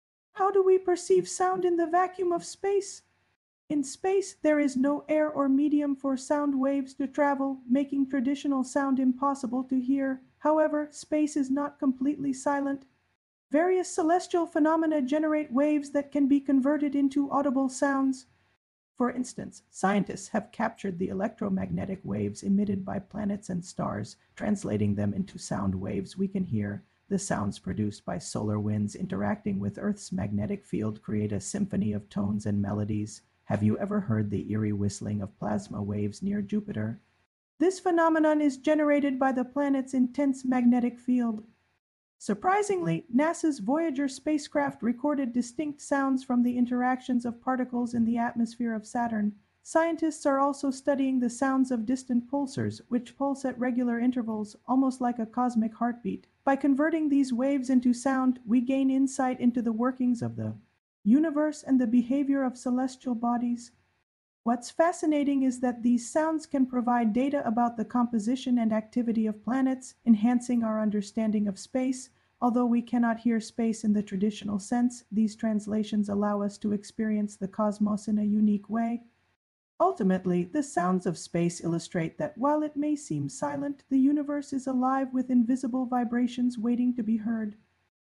The Sounds of Space Can You Hear Them
Explore the captivating sounds of space! Discover how celestial vibrations are transformed into audio experiences that connect us to the universe. Join us to hear the cosmic symphony!